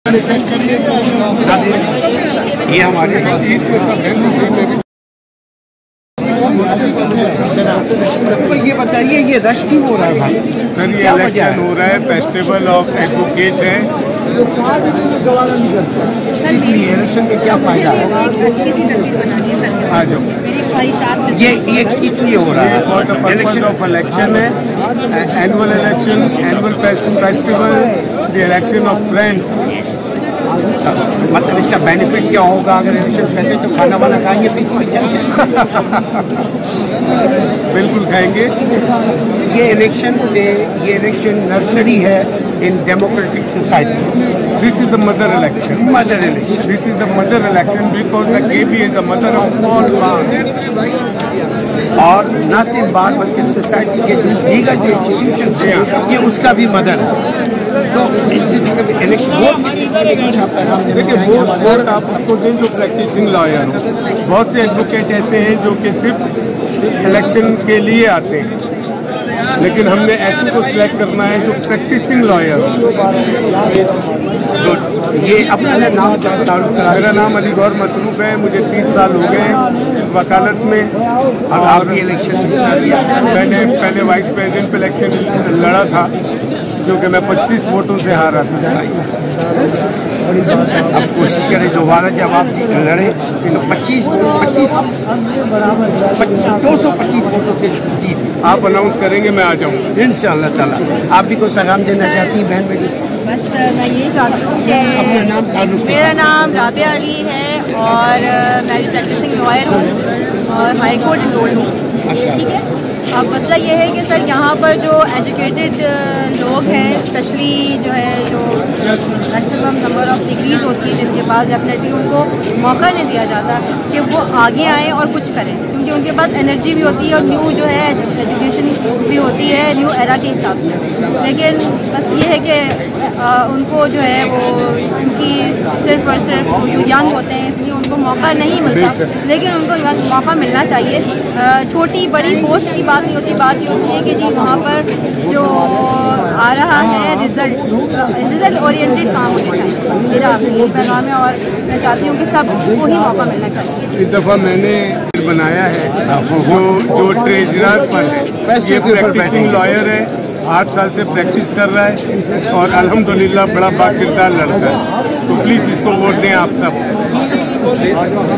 Adv Ali Gohar and Adv Miss Rabia Ali علی گوہر اور رابعہ علی
Adv Ali Gohar  Adv Rabia Ali Ki Bat Cheet Aslam Pervez Say Baat Cheet Karachi Bar Kay Hawaly Say Karachi Bar Election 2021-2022